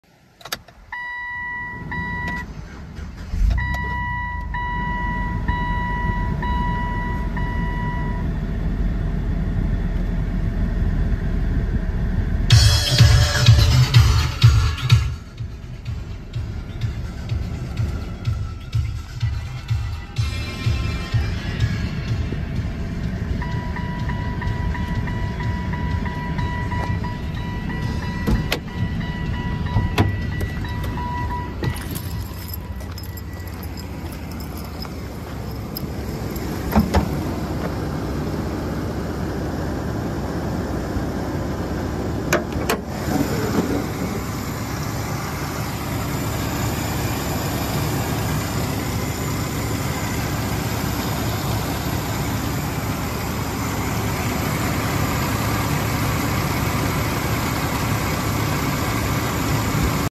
GovDeals: 2012 GMC Sierra 1500 Work Truck 2WD
2012 GMC Sierra 1500 Work Truck 2WD sold by Brewer Towing-Jacksonville Area Impound/Towing, FL on GovDeals.com